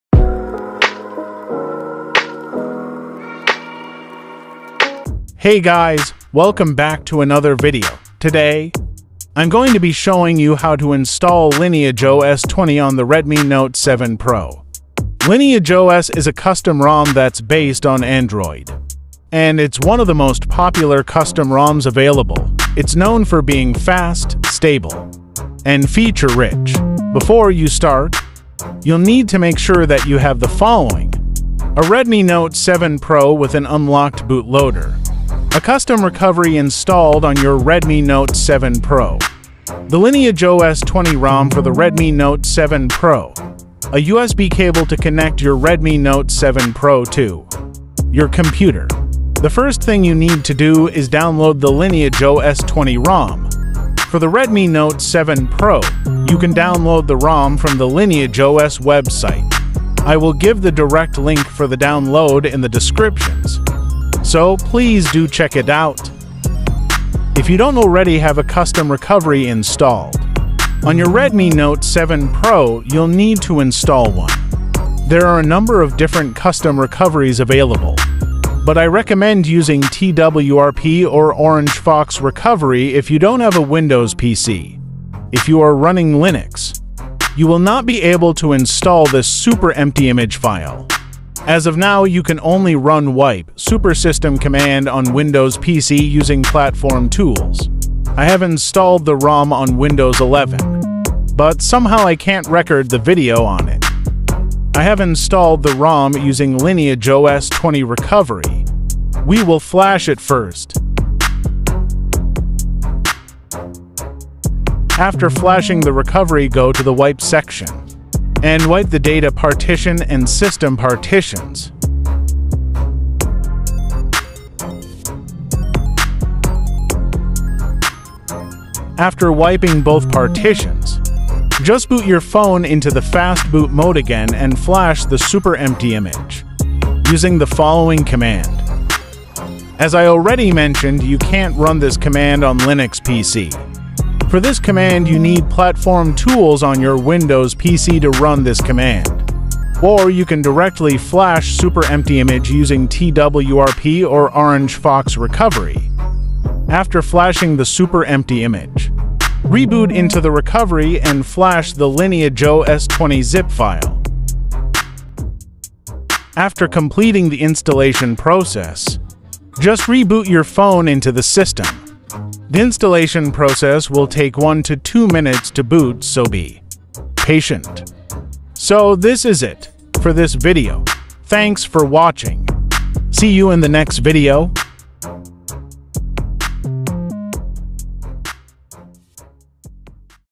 How To Install Lineage OS 20 on Redmi Note 7 Pro || Android 13
NOTE :- I am not able to record a screen on my Windows PC. The installation was done on Windows 11. Use Windows 10/11 PC to install this ROM or use custom recovery like TWRP or ORANGEFOX recovery.

Review of Lineage OS 20 for Redmi Note 7 Pro